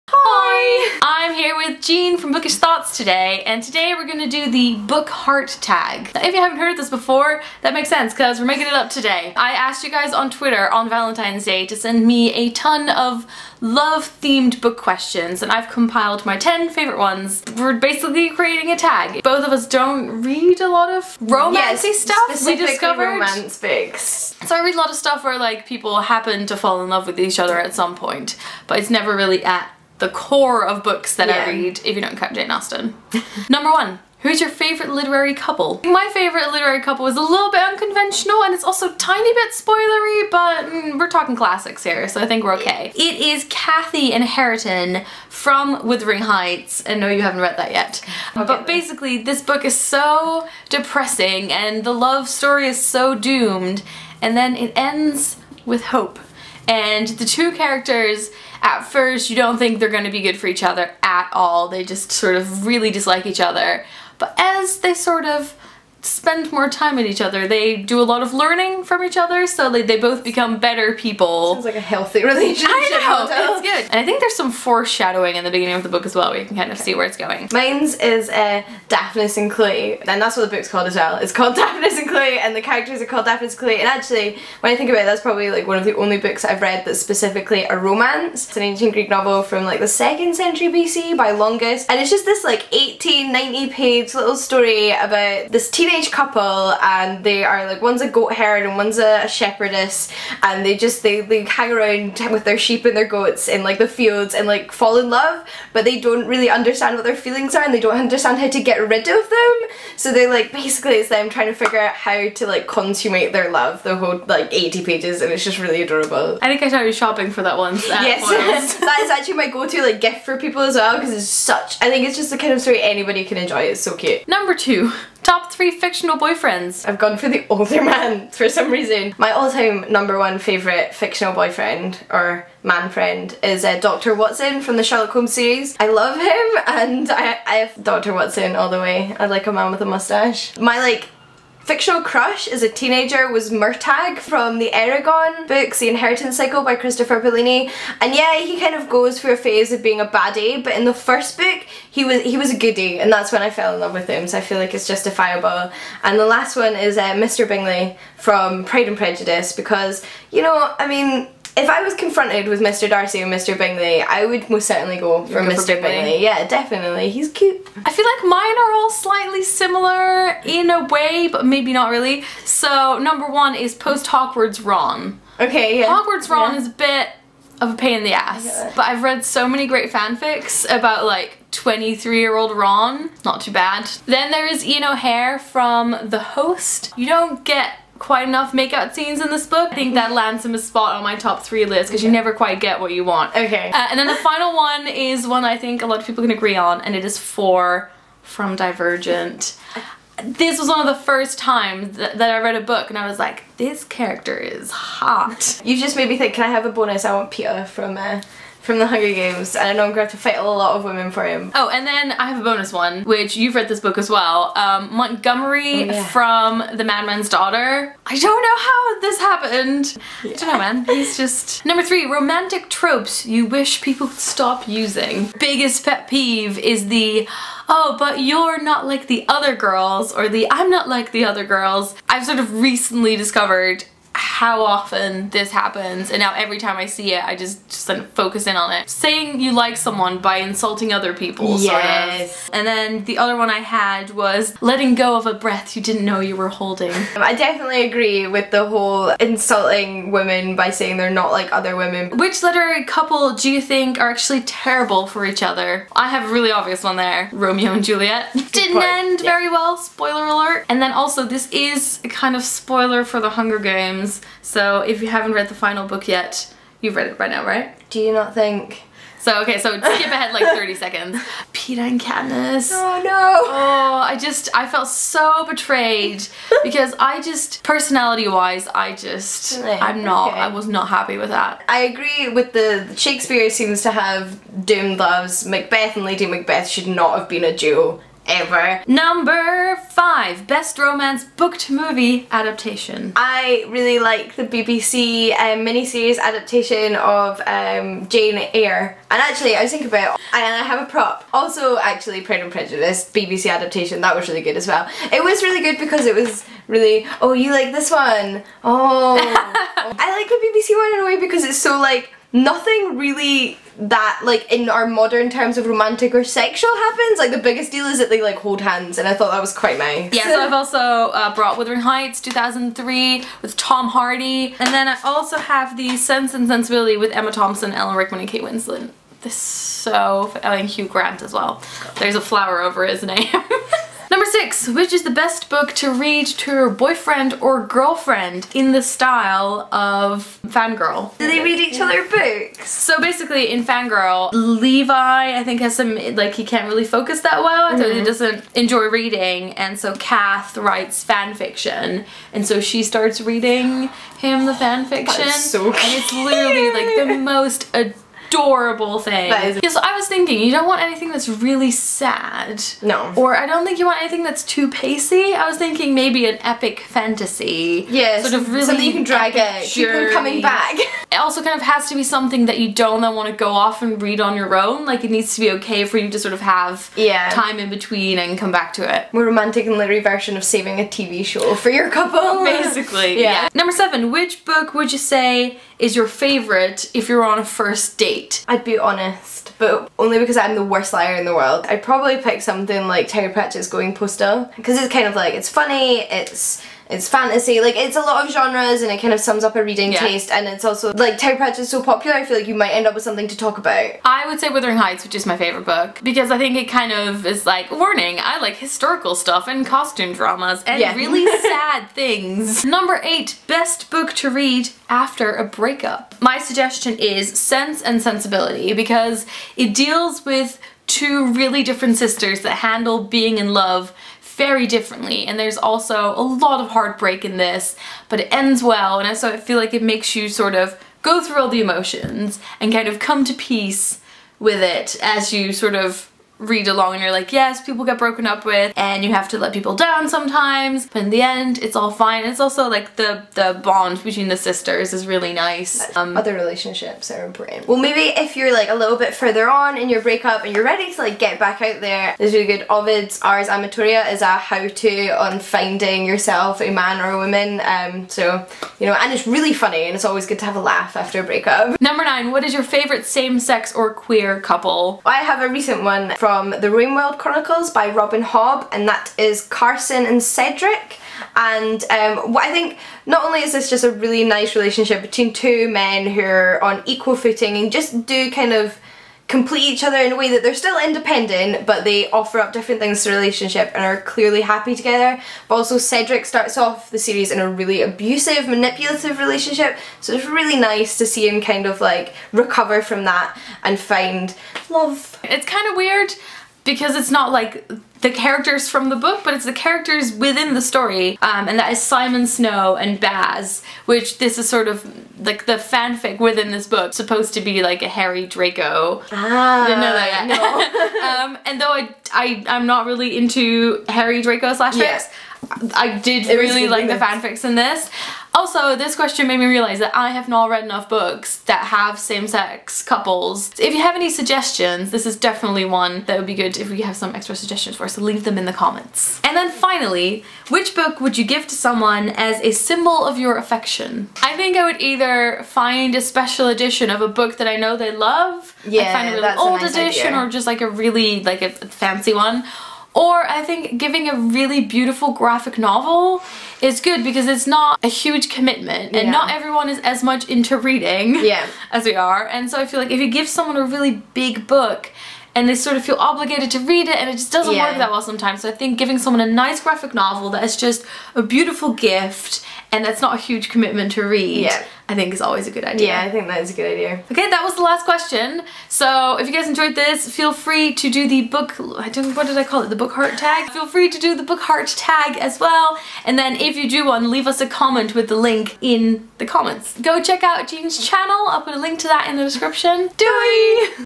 Book ♡ Tag with Jean.
Jean (bookishtoughts) and I answer your loved-themed book questions.

This video was filmed the week after Valentine's Day. Honestly I don't see how this is a surprise at this point.

1. Who is your favourite literary couple?
2. What's your Top 3 of fictional boyfriends/girlfriends?
3. What's a romantic trope you wish people would stop using?
4. Which literary couple are terrible for each other?
5. Best romance book to movie adaptation?
6. What is the best book to read to your boyfriend/girlfriend?
7. Which book would you say is your favourite on a first date?
8. Best book to read after a break up?
9. What is your favourite same sex or queer couple in literature?
10. Which book would you give to someone as a symbol of your affection?

Thanks to everyone who sent in a question. (I promise I had screenshots of all the tweets, but they were lost in the great MacBook meltdown of 2015.)

B O O K S

The Dragon Keeper (Rain World Chronicles) by Robin Hobb

Follow what I'm reading.